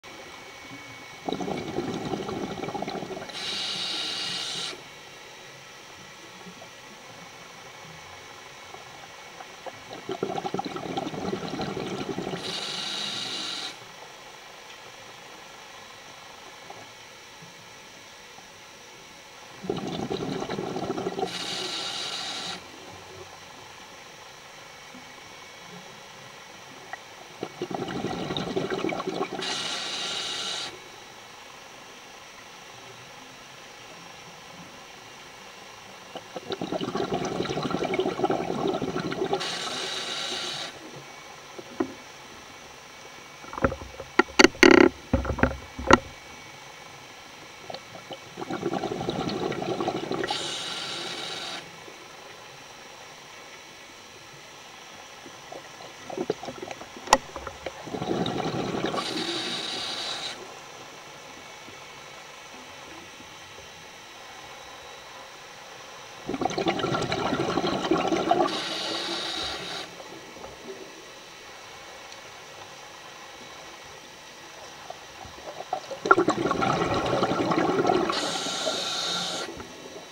Life on a wreck off Deal, Kent
Kentish orange anemones, oaten pipe hydroids and bib on a wreck off Deal, Kent